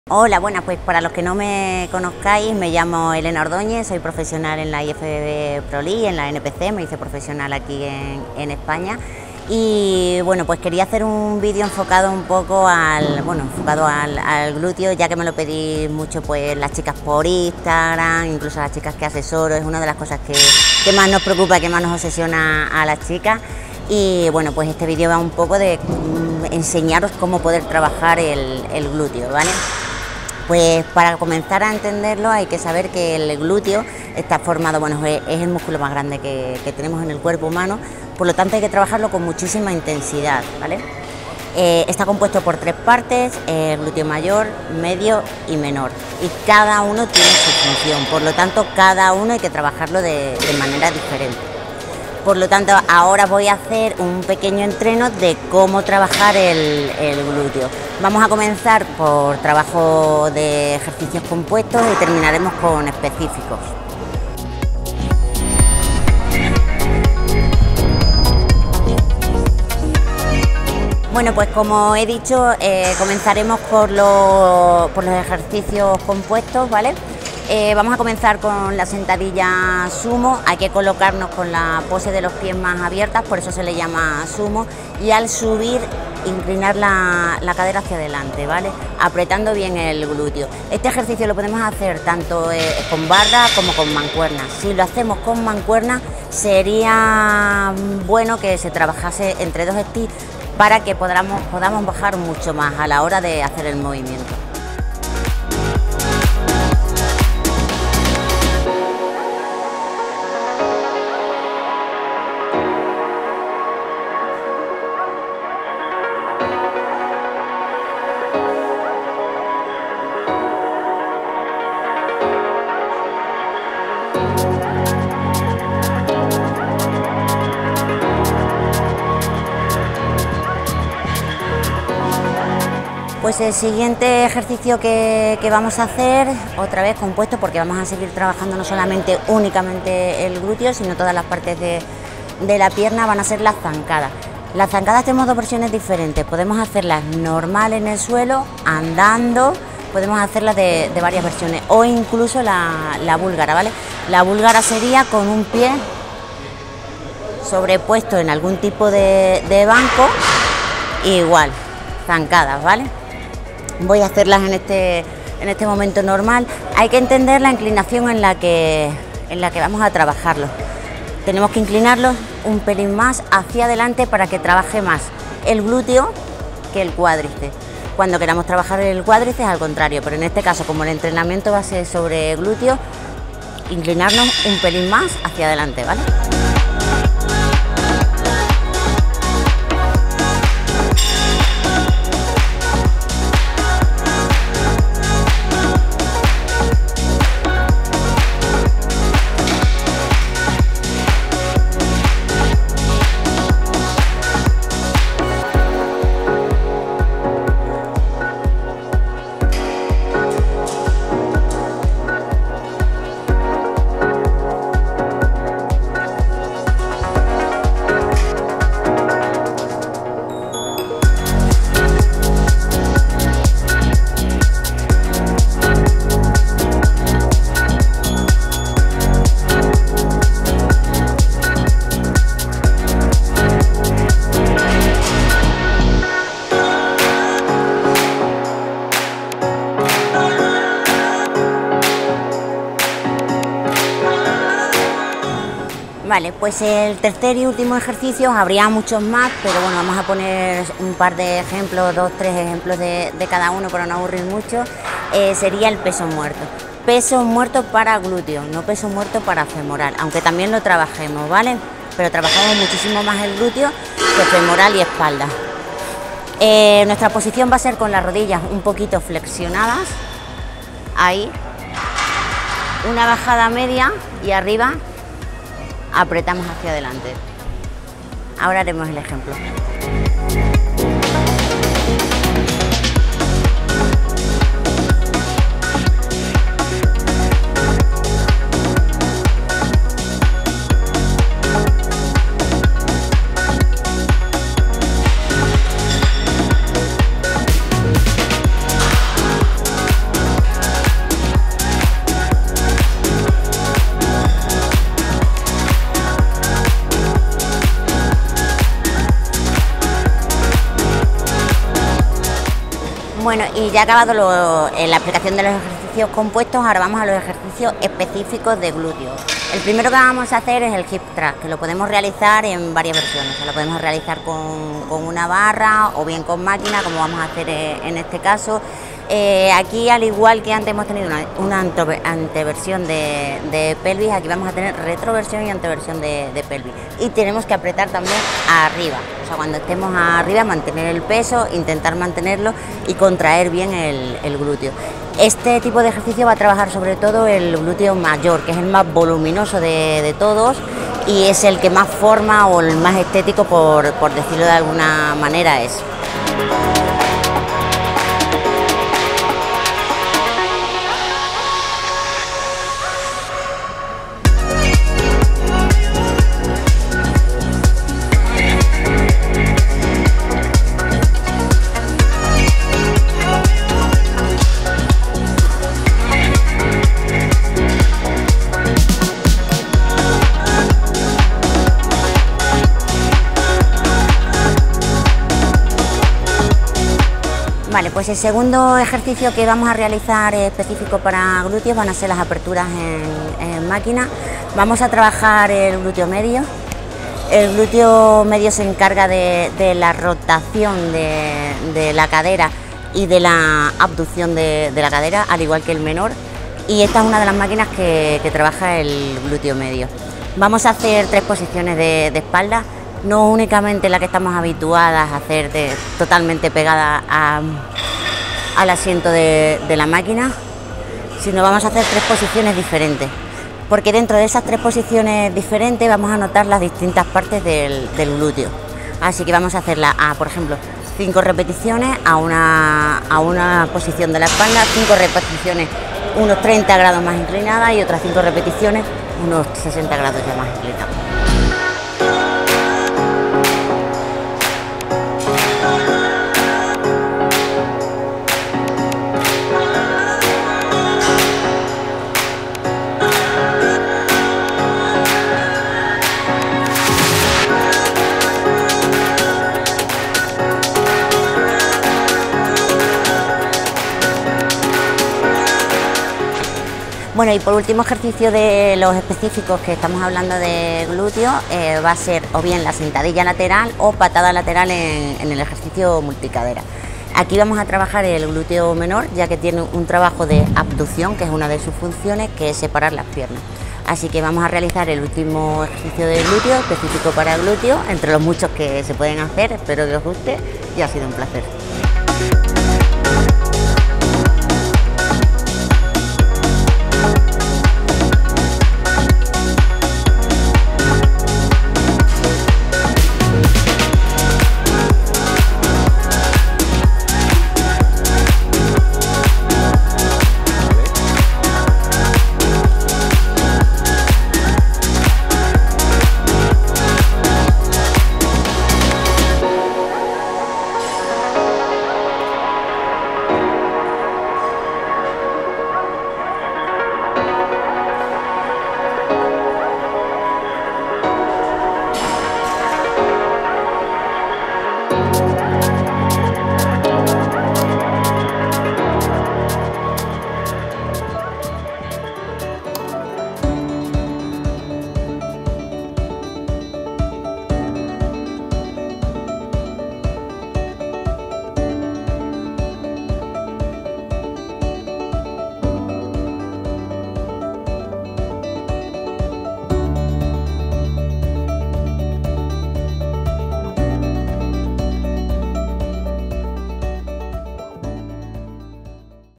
UNOS GLUTEOS PERFECTOS! 🍑 Los mejores ejercicios en el GYM con Helena Ordóñez
Los MEJORES EJERCICIOS para conseguir unos glúteos grandes y musculosos con Helena Ordóñez.

00:00 Inicio
01:11 Sentadilla sumo
02:13 Zancadas
04:05 Peso muerto para gluteo
05:13 Hip Thrust
06:25 Aperturas en máquina
07:48 Patada lateral